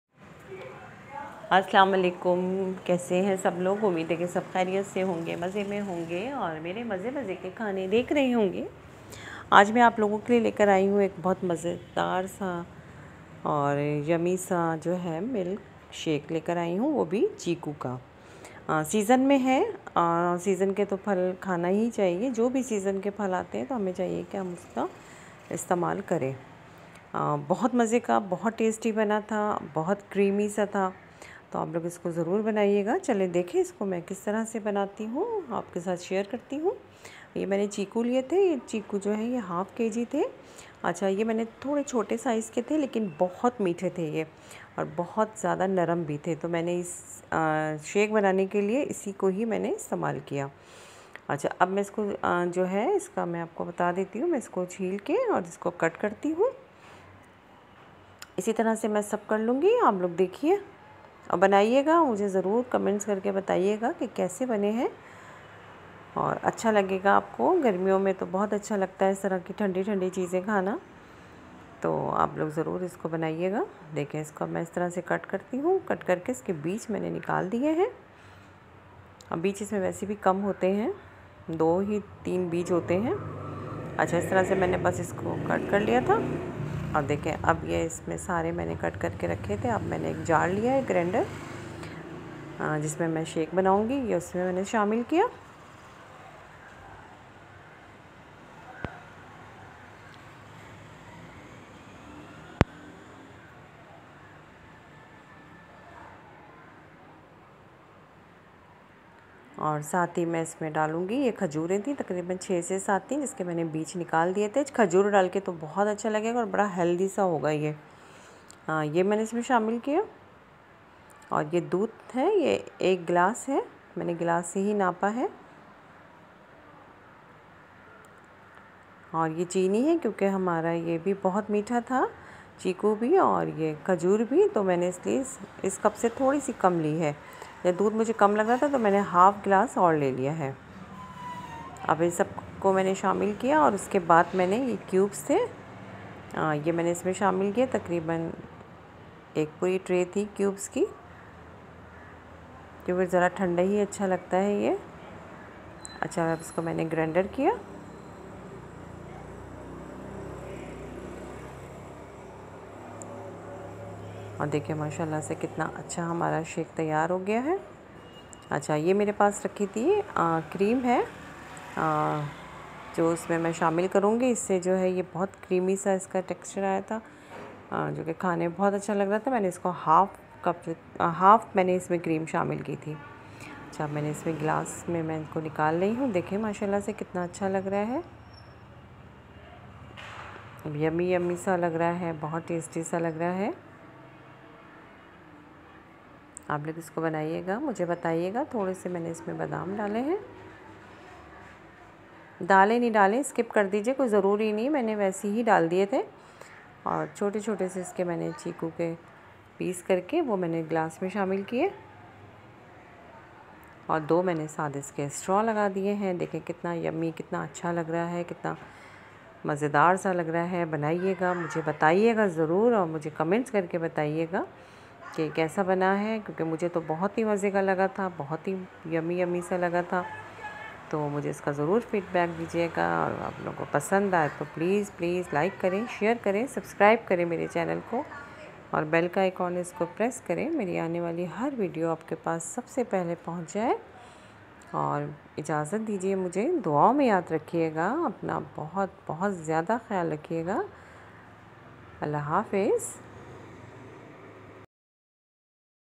Yummy Chikoo Milkshake || Summer special Milkshake || yummy desserts || Mazzy mazzy kay
How to make Chikoo Milkshake

if you enjoy my video like it comment on it and share with your family and friends try at home and give me feedback also

regards: Fouzia Khan